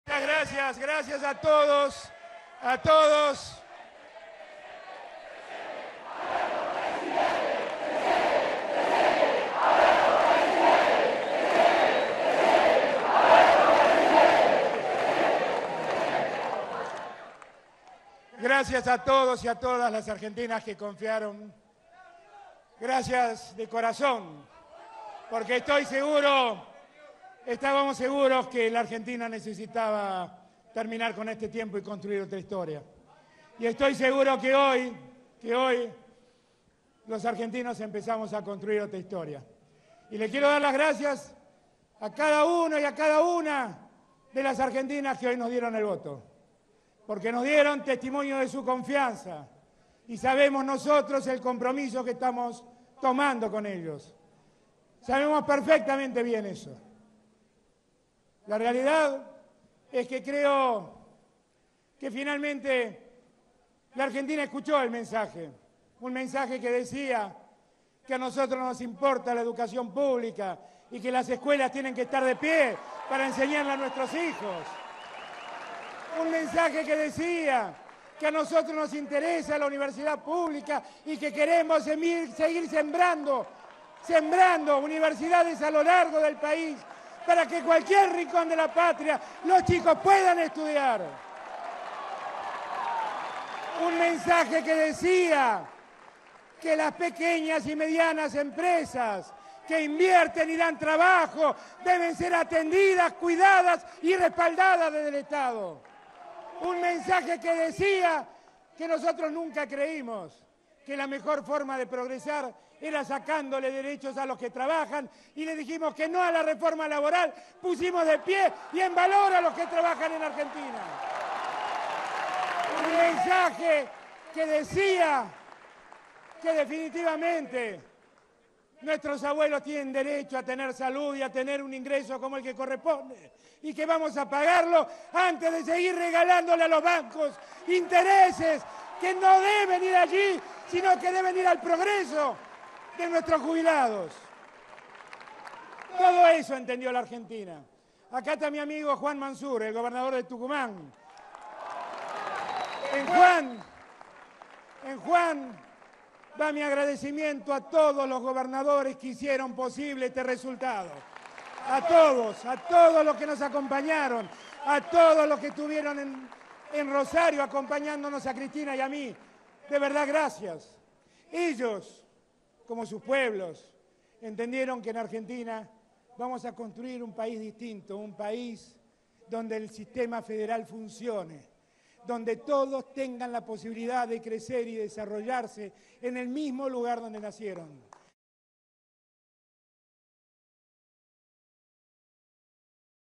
Alberto Fernández, tras imponerse en las PASO: "Se lo debo a Estanislao"
El momento en el que Alberto Fernández se abrazó con su hijo Estanislao, tras imponerse en las PASO: "Se lo debo a Estanislao"